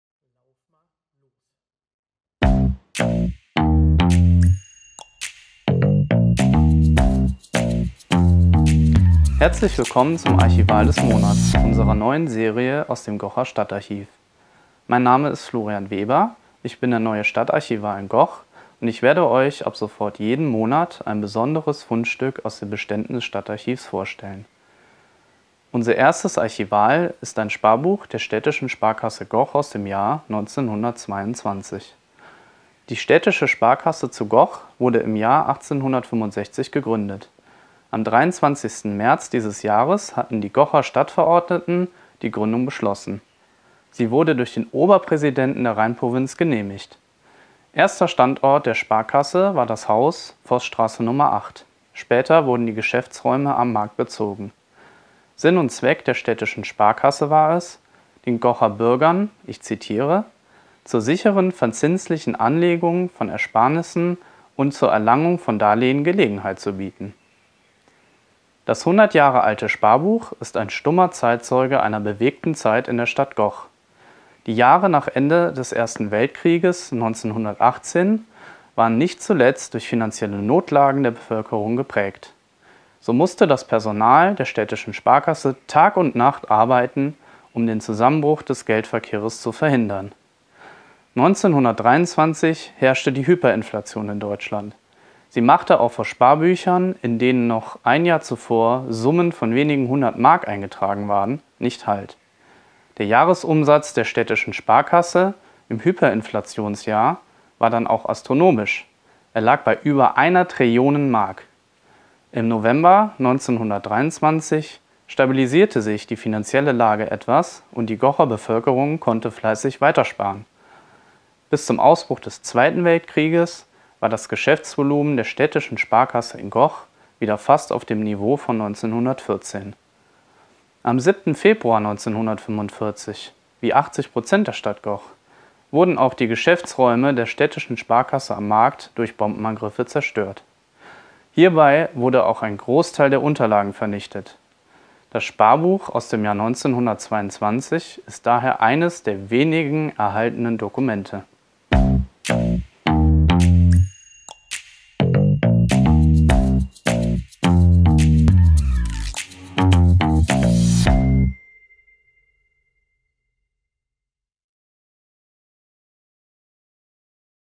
Archivale des Monats Juni 2022
Folge 1 unserer neuen Serie aus dem Stadtarchiv. Stadtarchivar Florian Weber sucht ab sofort jeden Monat einen besonderen Gegenstand für euch aus dem Bestand heraus und berichtet, was es damit auf sich hat. Heute geht es um ein Sparbuch, das vor genau 100 Jahren eröffnet worden ist.